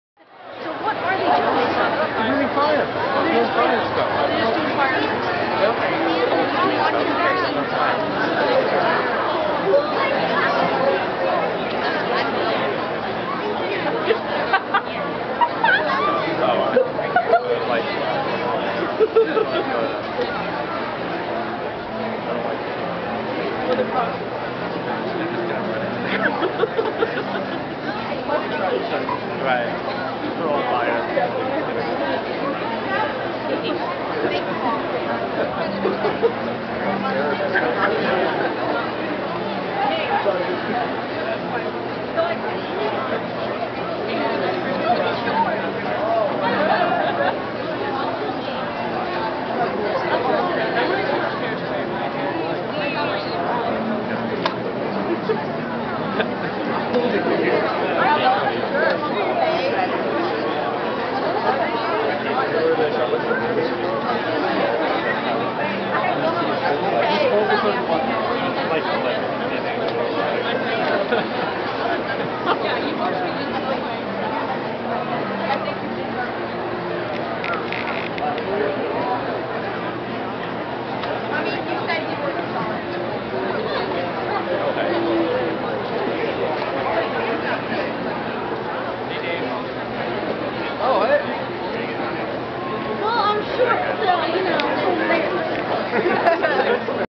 Bangor Fire Dancers at Diagon Alley
Downtown Bangor was transformed into Diagon Alley for the  Harry Potter Midnight Magic Party and the Bangor Fire Dancers added to the mystique well.

7/31/2016